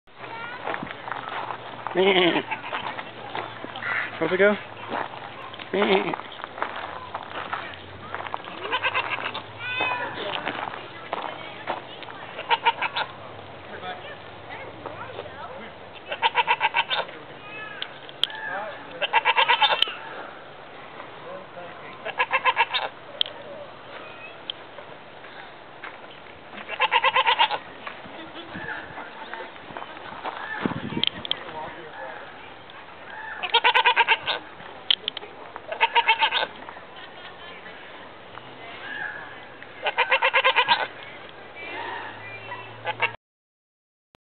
Talking Mini Goat.AVI
A talking mini goat I shared a brief conversation with while visiting bearizona.